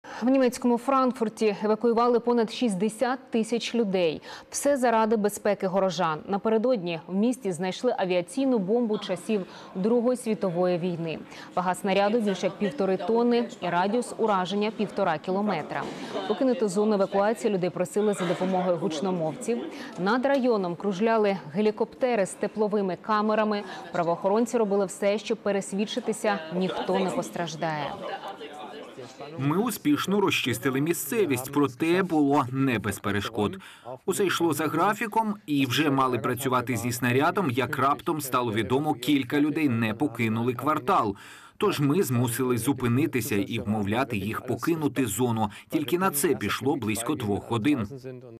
У німецькому Франкфурті евакуювали понад 60 тисяч людей
Усе – заради безпеки горожан. Напередодні у місті знайшли авіаційну бомбу часів Другої Світової війни. Вага снаряду – більш як півтори тонни й радіус ураження півтора кілометра. Покинути зону евакуації людей просили за допомогою гучномовців. А над районом – кружляли гелікоптери з тепловими камерами. Правоохоронці робили все, щоб пересвідчитися – ніхто не постраждає.